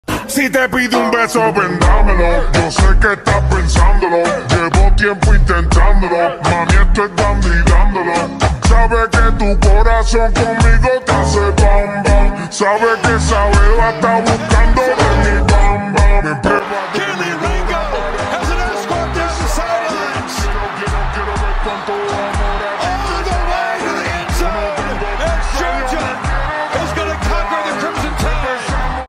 GEORGIA NATIONAL CHAMPIONSHIP 🏈🔥
This is a compilation of photos from the previous national championship.